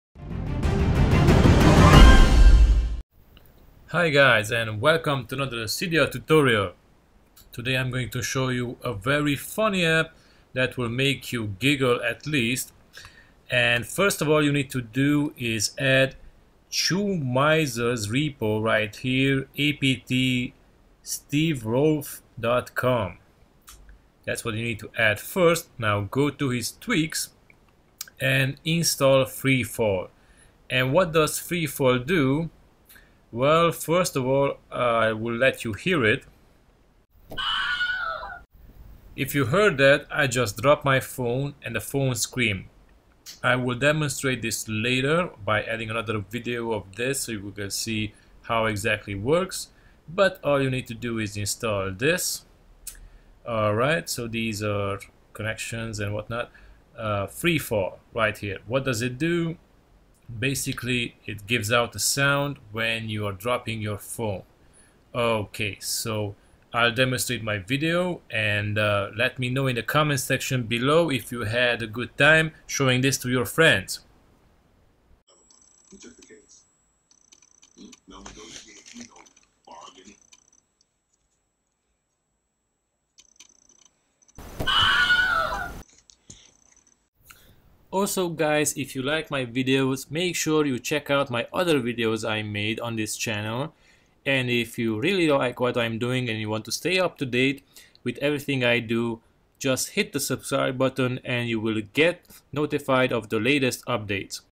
Freefall - Make your iPhone scream | Cydia iOS 8.4 Jailbreak tweak
Freefall is a fun Cydia Jailbreak tweak that will make your iPhone scream. But be careful when you drop your phone.